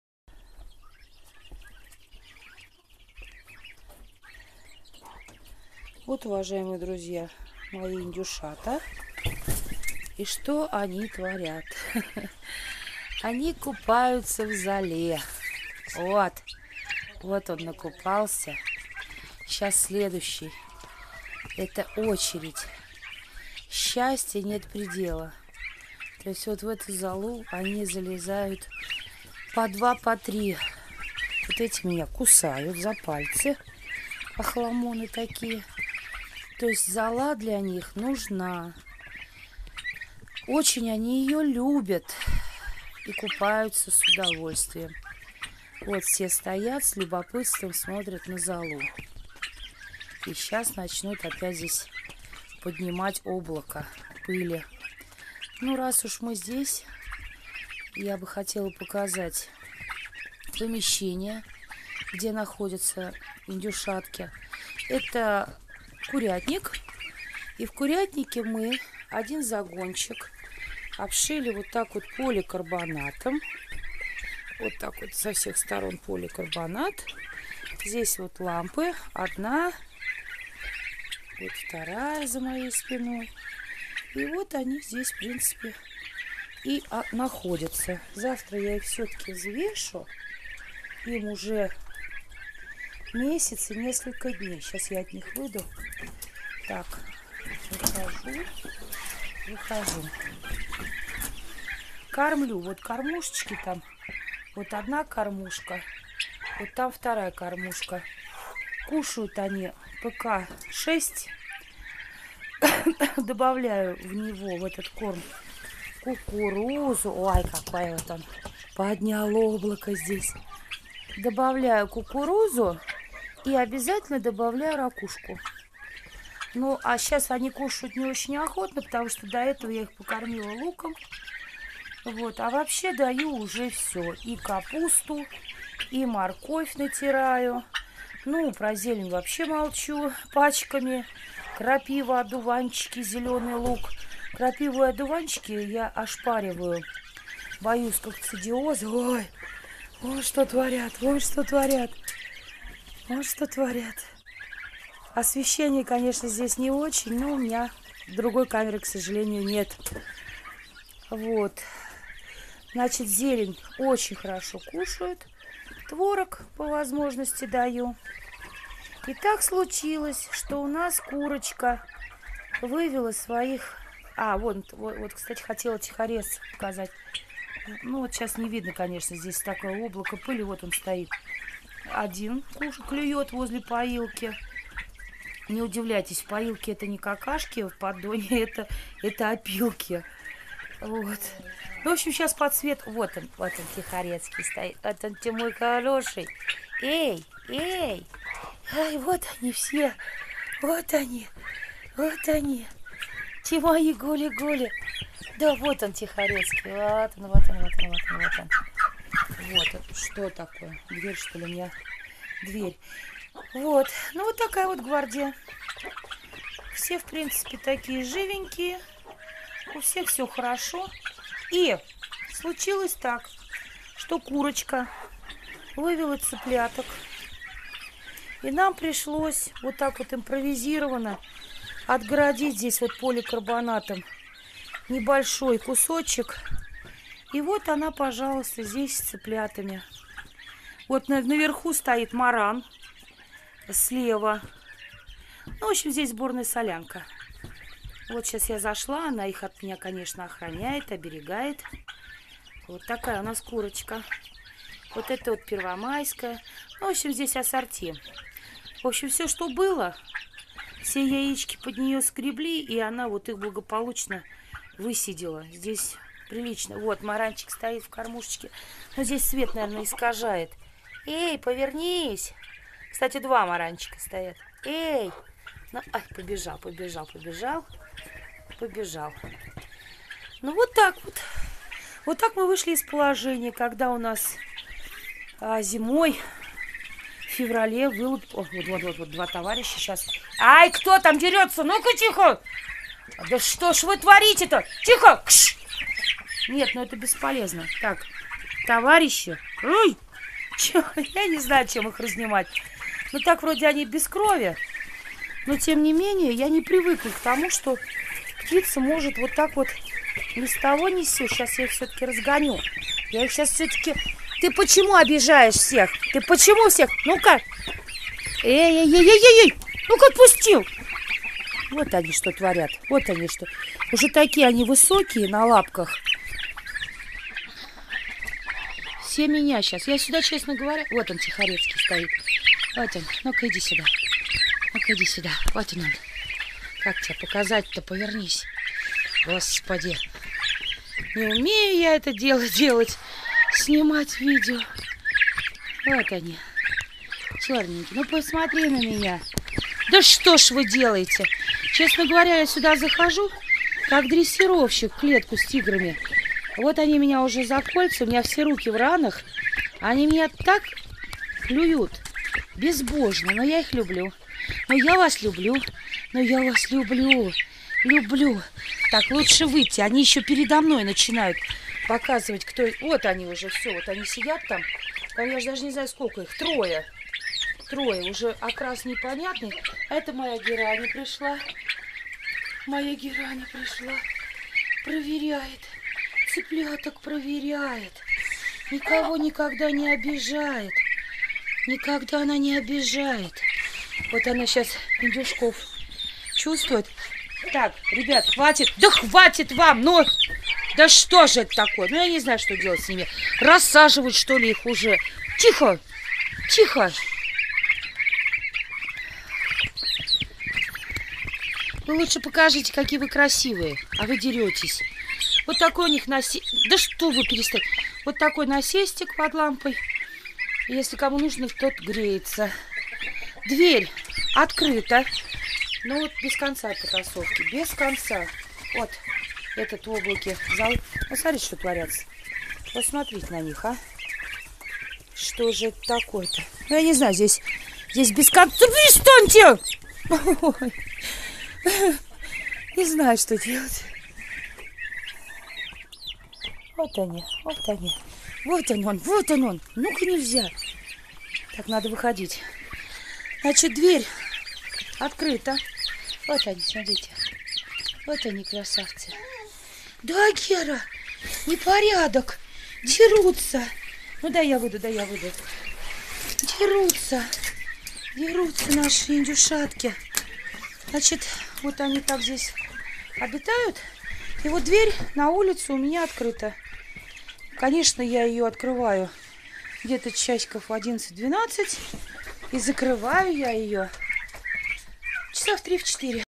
Индюки.Индюшатам из Ставрополья 1 месяц
Индюки.Индюшатам 1 месяц, чем кормить,пропаивать,условия содержания.